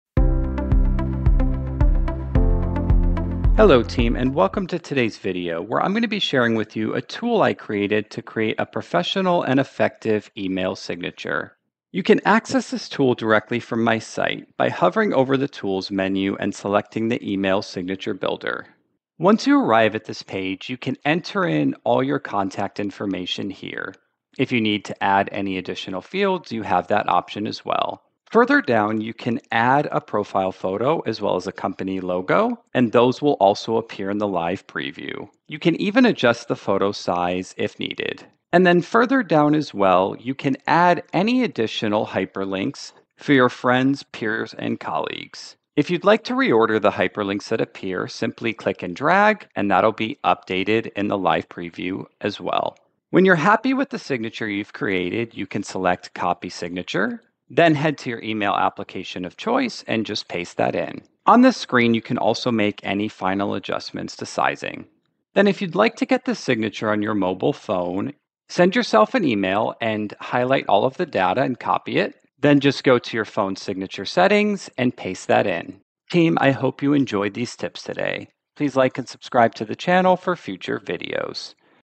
Career Tool - Free Custom Professional Email Signature Generator | Mobile Enabled | Outlook and More
Hey team,

In this video we cover a free tool I created to help you create custom, professional, and engaging email signatures. These can be used on both PC and mobile devices.